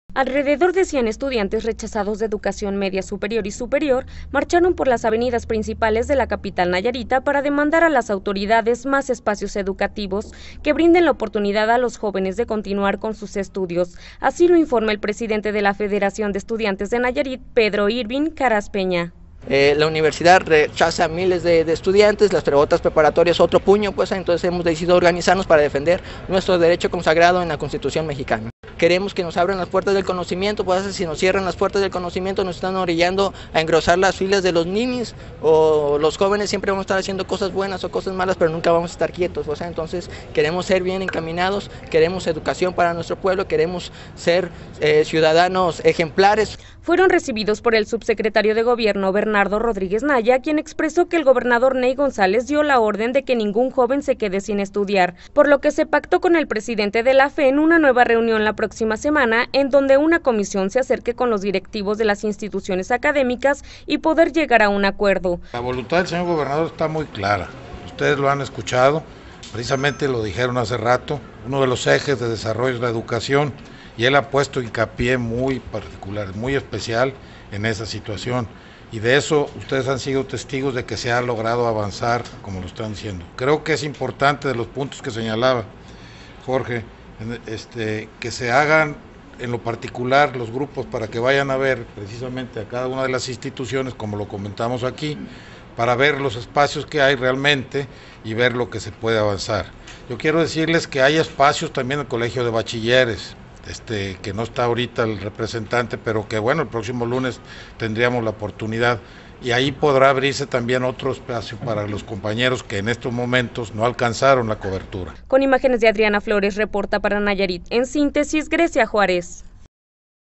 RECHAZADS EXIGEN ESPACIOS EN ESCUELAS PUBLICAS
ESTUDIANTES RECHAZADOS PARA INGRESAR A LAS ESCUELAS PÚBLICAS DEL NIVEL MEDIO SUPERIOR Y SUPERIOR MARCHARON POR LAS CALLES DE TEPIC PARA EXIGIR ESPACIOS EN EL CETIS 100, CONALEP, CECYTEN Y OTROS PLANTELES DE EDUCACIÓN PARA EL CICLO ESCOLAR QUE ARRANCA EL PRÓXIMO 22 DE AGOSTO.